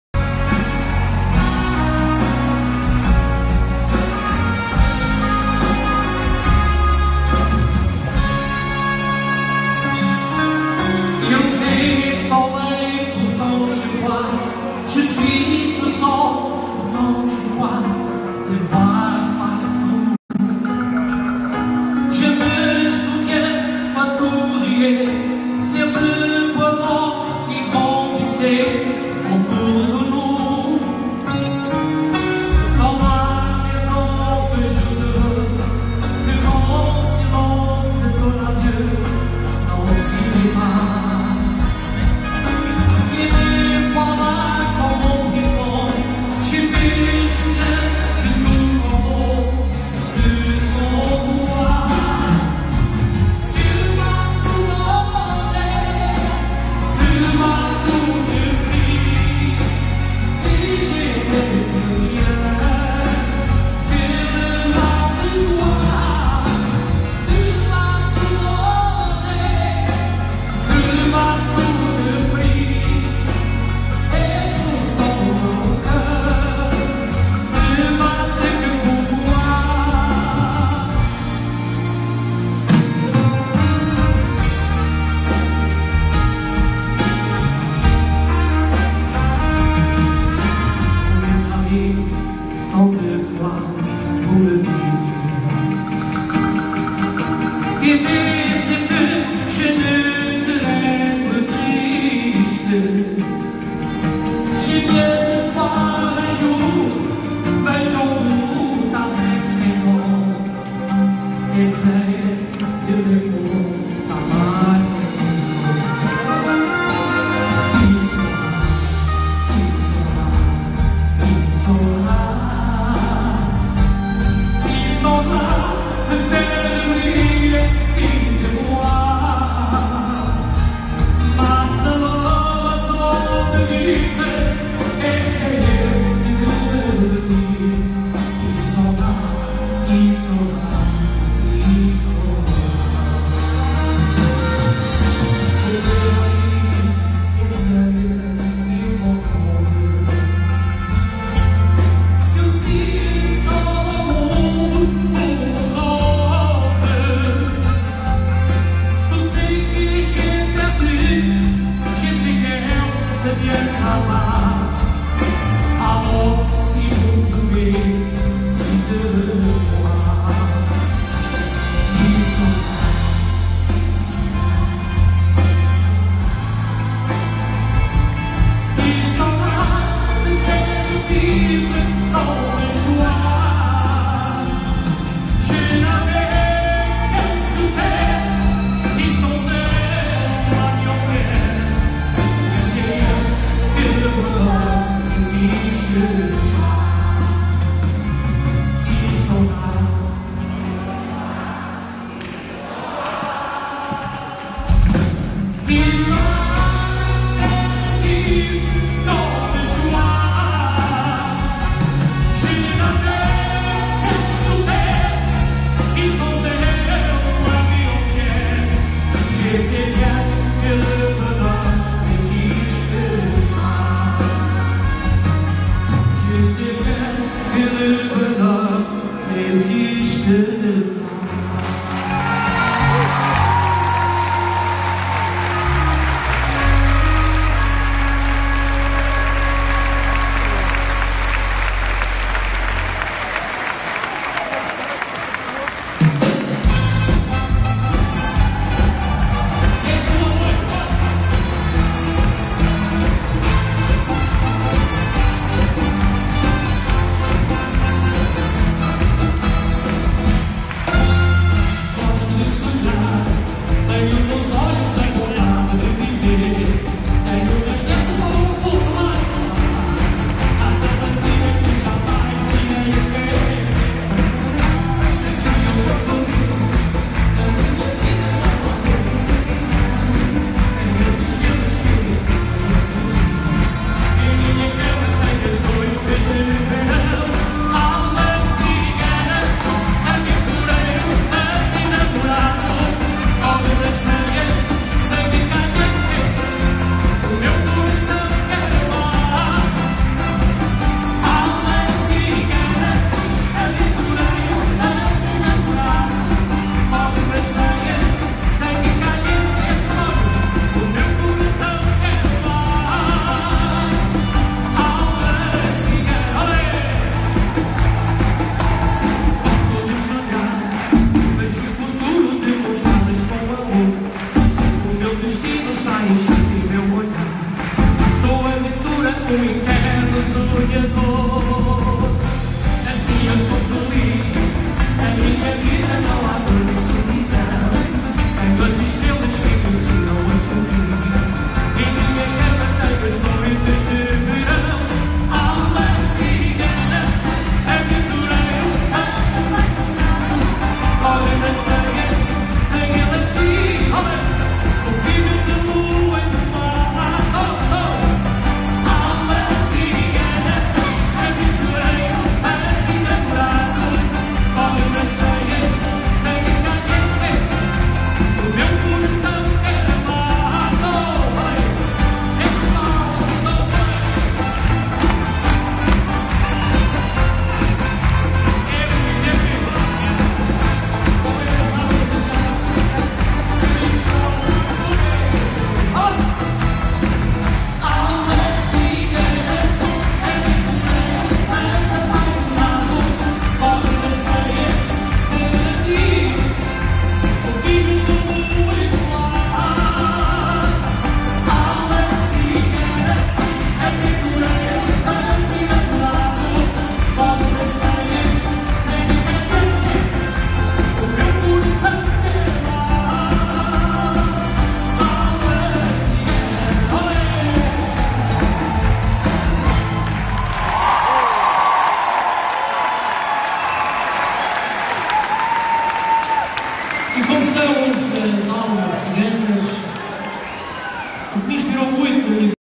Luis Filpe Reis no Coliseu do Porto 2018 : Nexidancer
Corpo de baile Nexidancer
Nexah Brito
Aide Queiroz
Edu Holanda
Luis Filipe Reis no Coliseu do Porto 2018